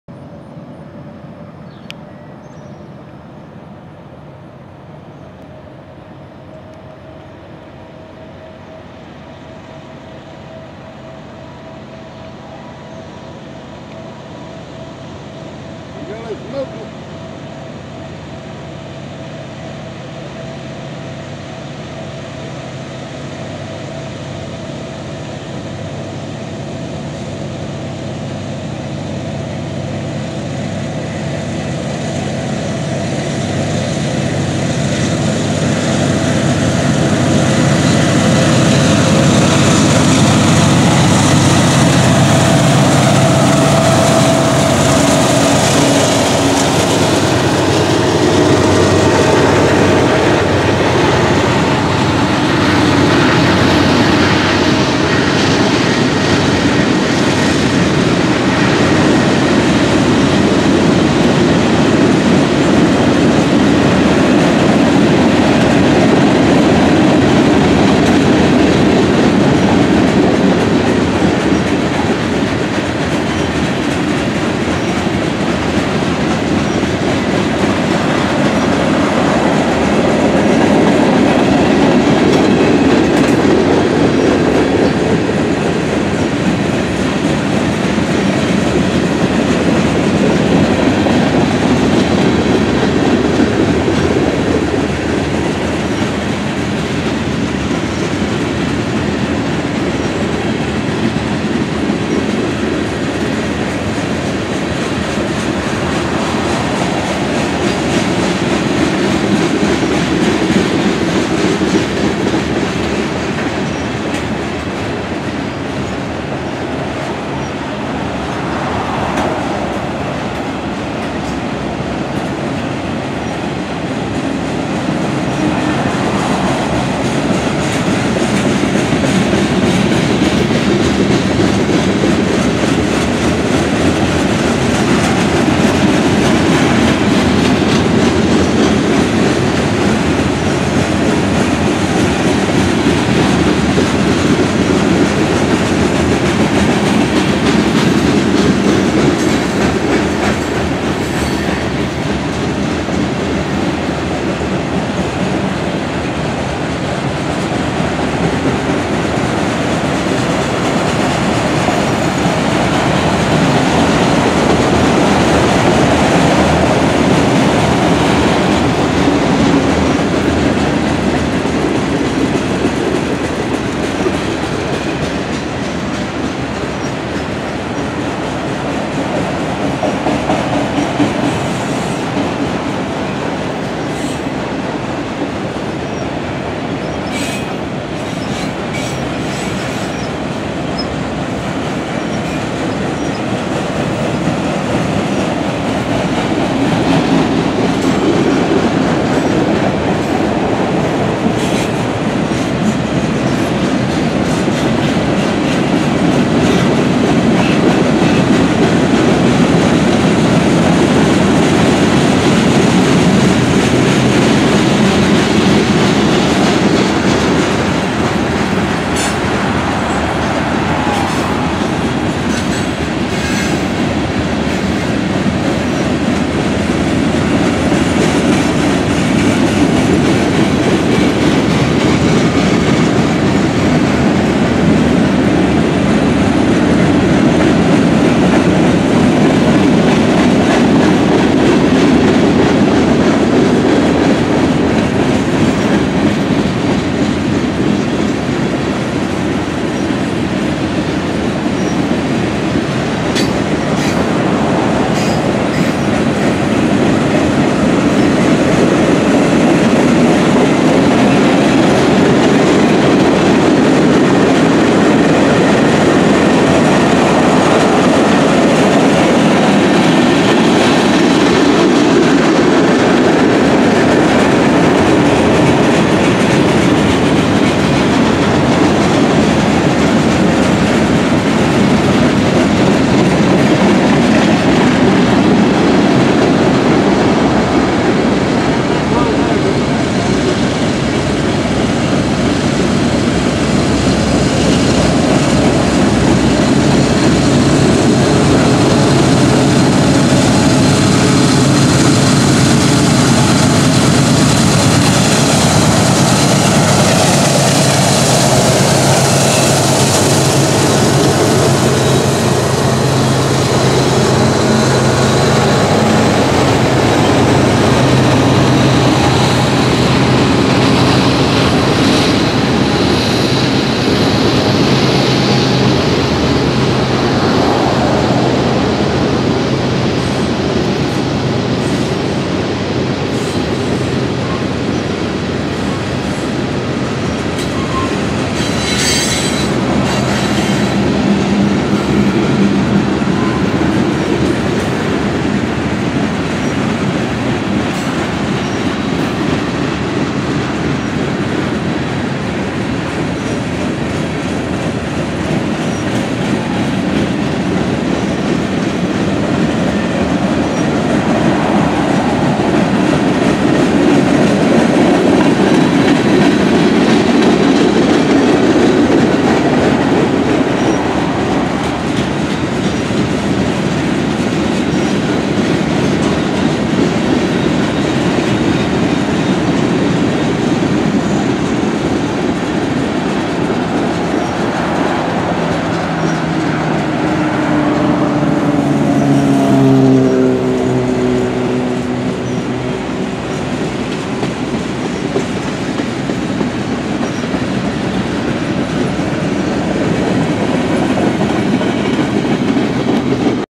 Train 126 7/13/21. Bristol, TN. Has 8 EX-BNSF engines.
Eastbound! Train 126 7/13/21 on Tuesday in Bristol, Tennessee! Has 8 EX-BNSF Engines.